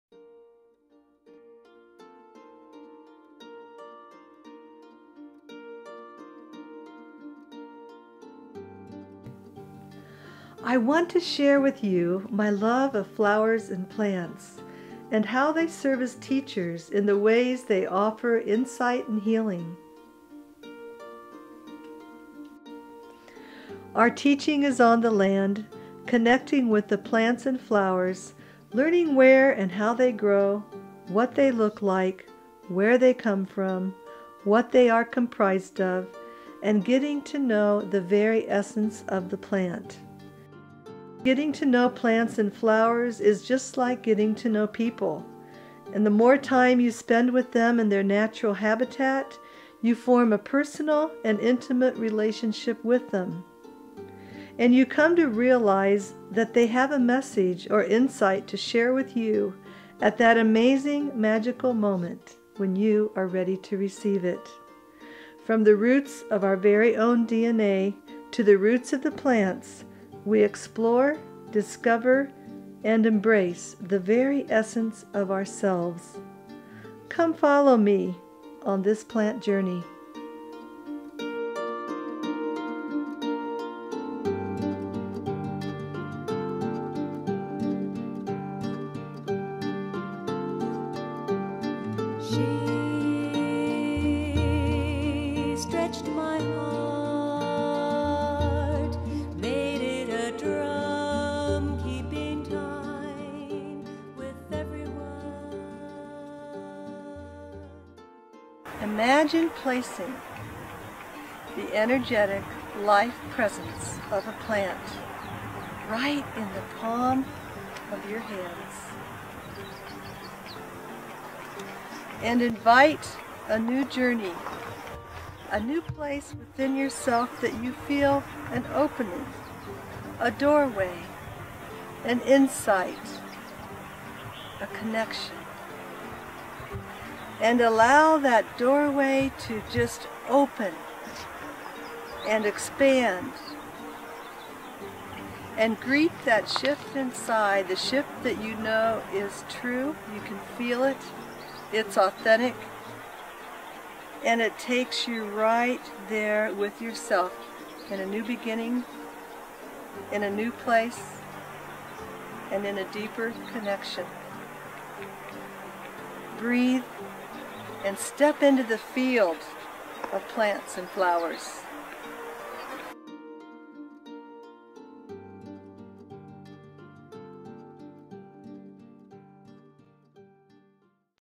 Intro.mp4
Rhonda PallasDowney is the founder of Living Flower Essences and the Center for Plant Studies and Healing Arts.  Author of The Healing Power of Flowers, and Voices of Flowers.